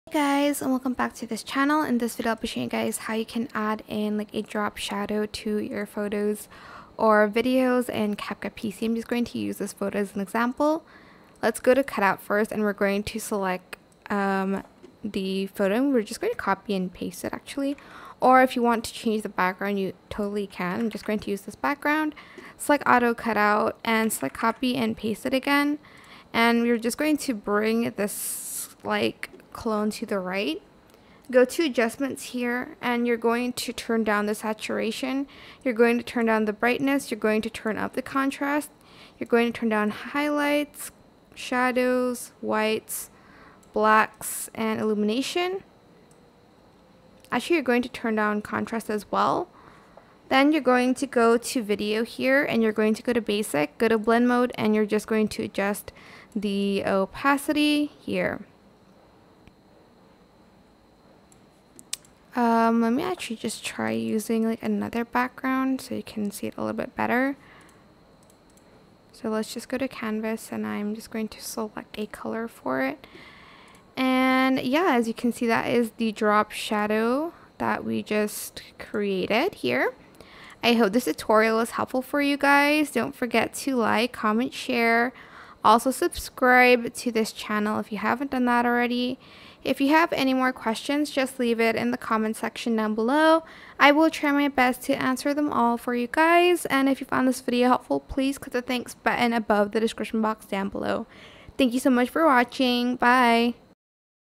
👍 INSTANT HELP: Drop Shadow On Object Tutorial In CapCut PC! Create This Fake Shadow
Learn how you can create this drop shadow on CapCut PC by watching this video.